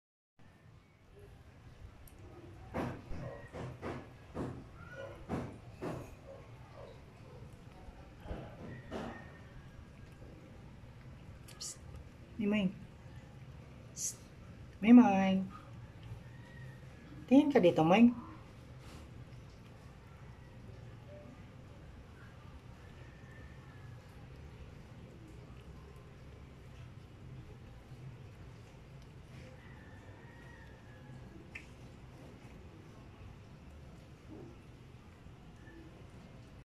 FEEDING ADOPTED STRAY CAT
This is a stray cats that i found in the street, And look so hungry thats why i decided to bring it yo my house and adopt.